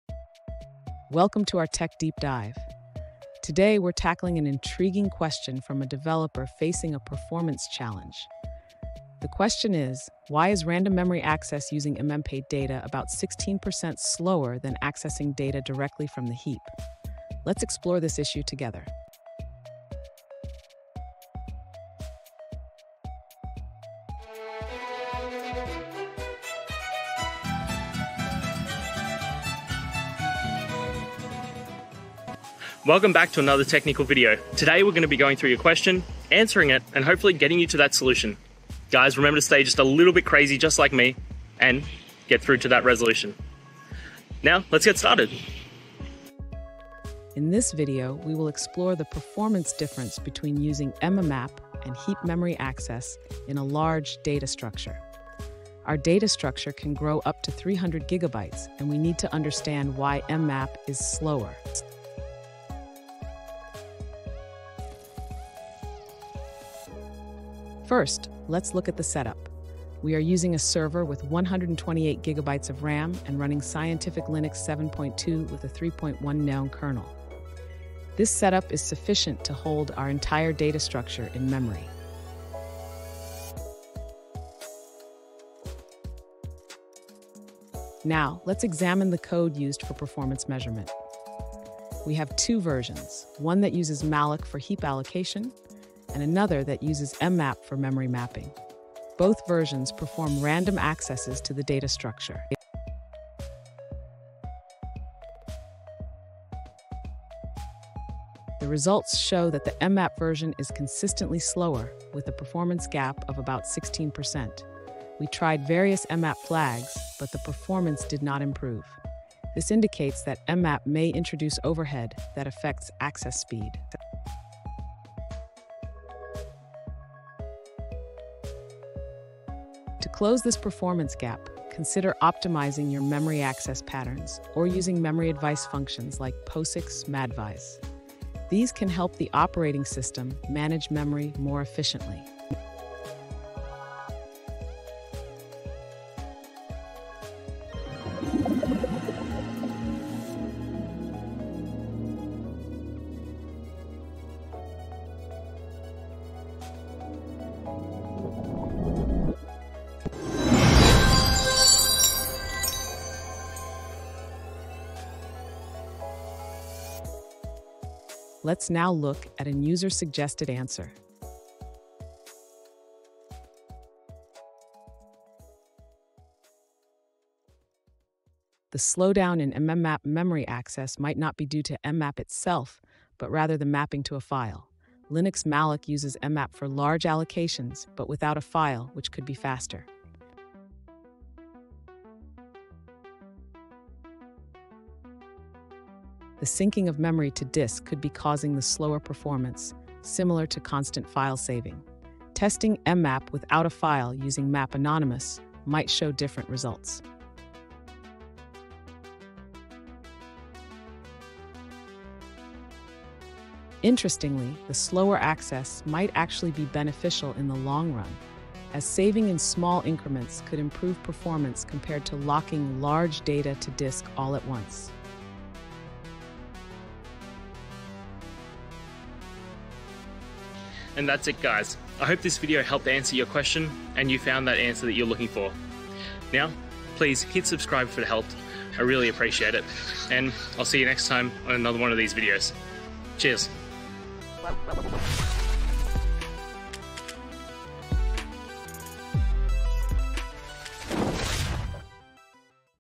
Why Random mmaped Memory Access is 16% Slower than Heap Data Access
In this video, we delve into the intriguing world of memory access patterns, specifically comparing random mmaped memory access to traditional heap data access. While mmaped memory offers unique advantages, our analysis reveals that it can be significantly slower—by as much as 16%. Join us as we explore the underlying reasons for this performance gap, examining factors such as caching behavior, memory locality, and the implications for application performance. Whether you're a developer or a tech enthusiast, this discussion will enhance your understanding of memory management in modern computing.

Today's Topic: Why Random mmaped Memory Access is 16% Slower than Heap Data Access

Related to